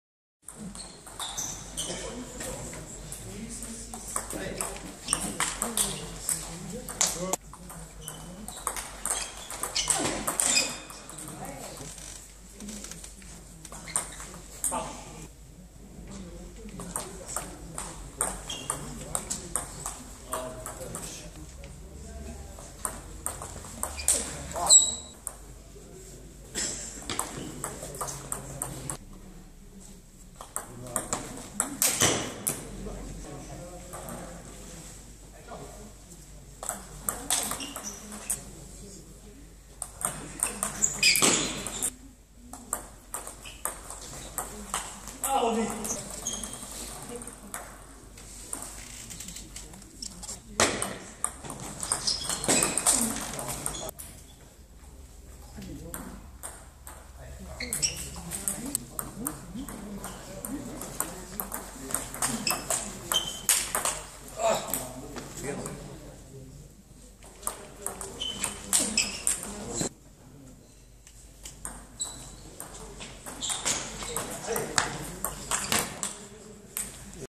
TENNIS DE TABLE (Nat. 3) A.T.T. Le Havre 3 - 8 Argentan Bayard
TENNIS DE TABLE (Nat. 3, 2è phase, J.2 - S.30 janv. 2016) A.T.T. Le Havre 3 - 8 Argentan Bayard

Double Cornel Stoica - Nicolas Commare et David Robillard - Lucas Leconte (2/2)